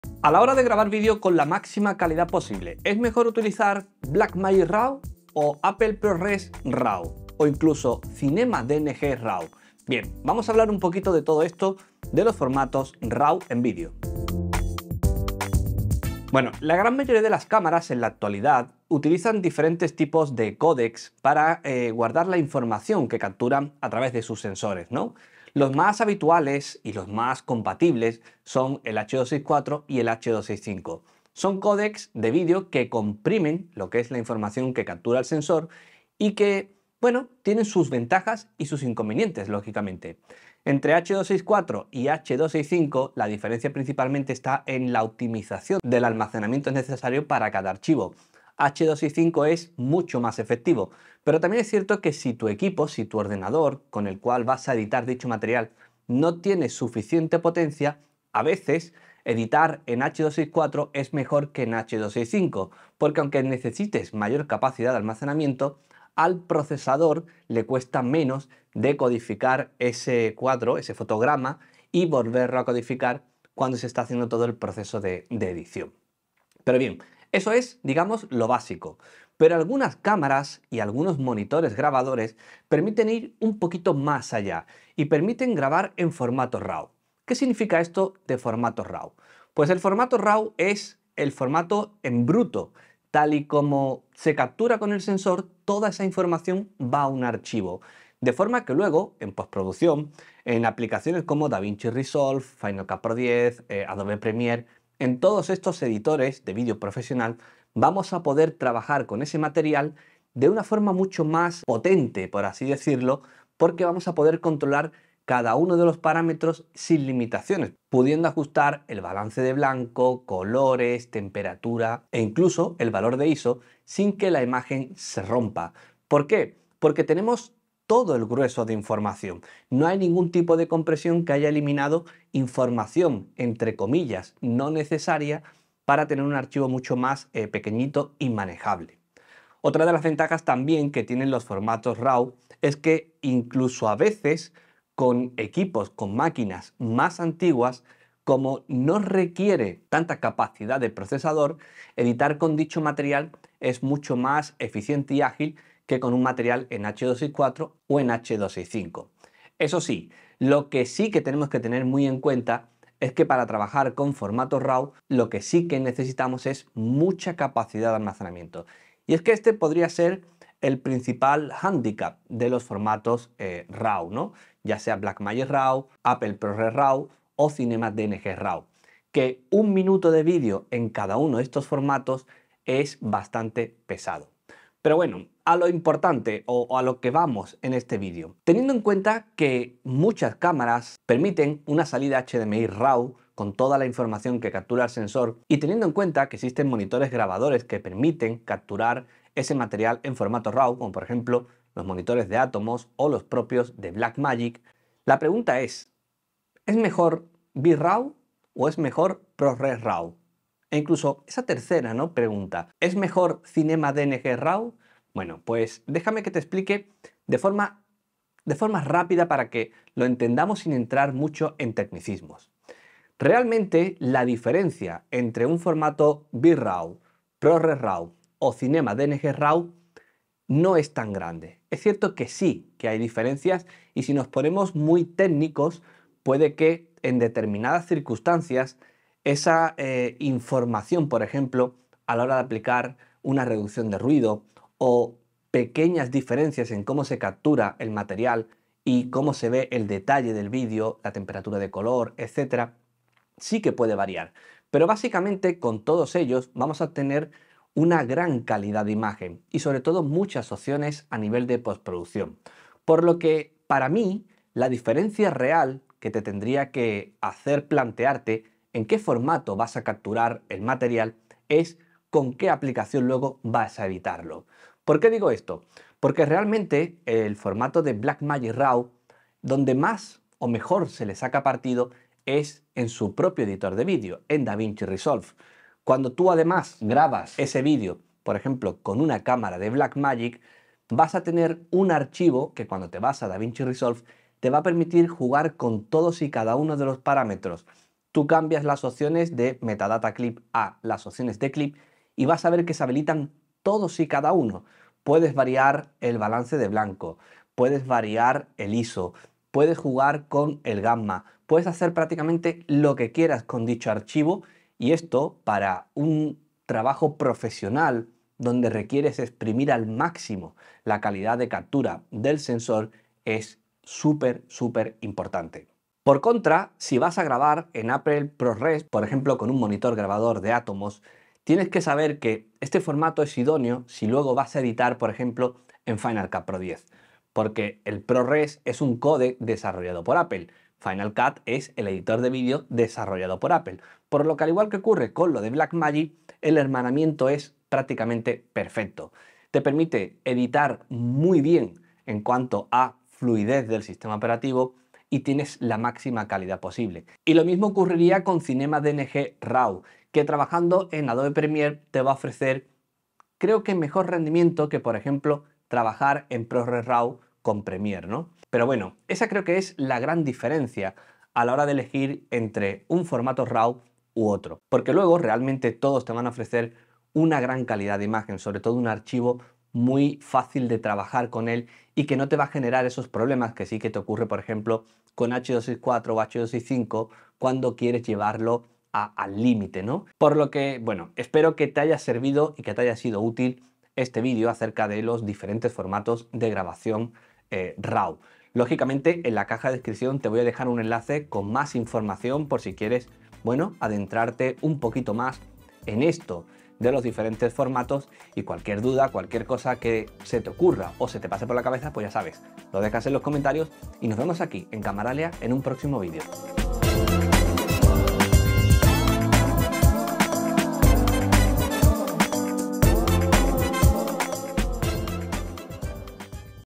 🎥 Guía Completa de Formatos RAW para Vídeo: TODO lo que Necesitas Saber
Descubre qué son, para qué sirven y cuándo debes usarlos para obtener la mejor calidad de imagen en tus producciones.  Más información 🔽🔽

Formato RAW, Vídeo, Grabación de Vídeo, ProRes RAW, CinemaDNG, Calidad de Imagen, Producción Audiovisual, Edición de Vídeo, Cine Digital.